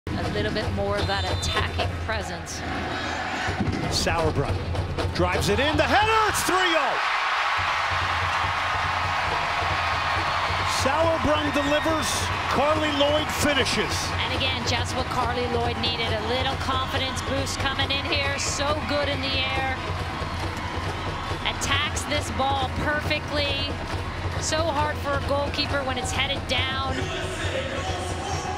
WNT vs. Costa Rica: Carli Lloyd Goal - July 22, 2016
Carli Lloyd heads down the free kick from Sauerbrunn right into the corner for her 88th career goal.

U.S. Soccer